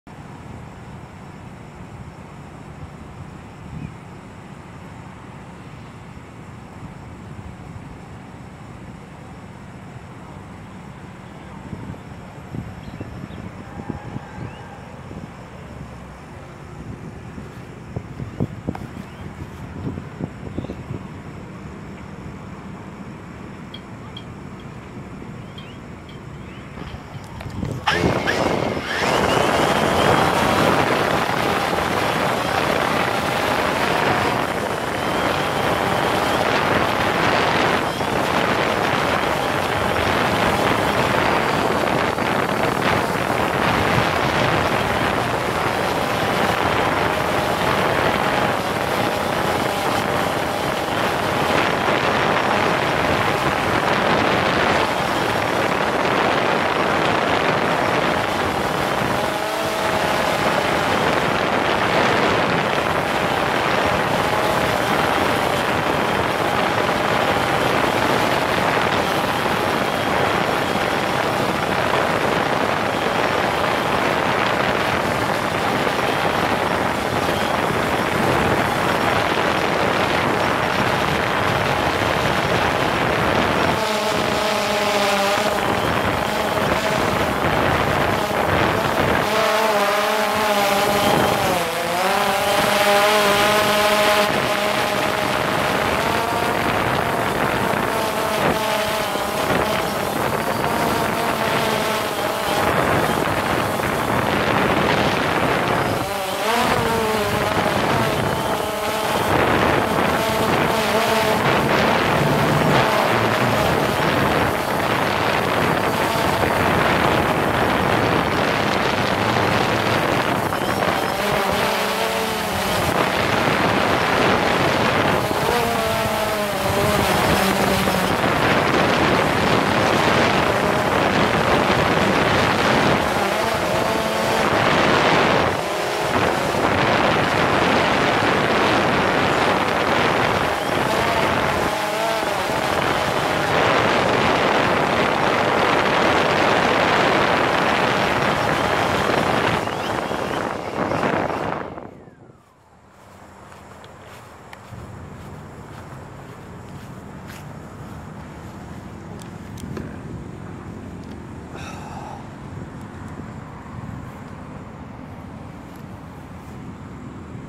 360 4k Camera Test on Drone | Drag the screen to look around!
Samsung 360 camera on DJI Phantom 3 Standard. Drone had trouble stabilizing for wind and the 360 cam's weight. Still, better than the first test which ended in a nasty crash and was an abysmal failure.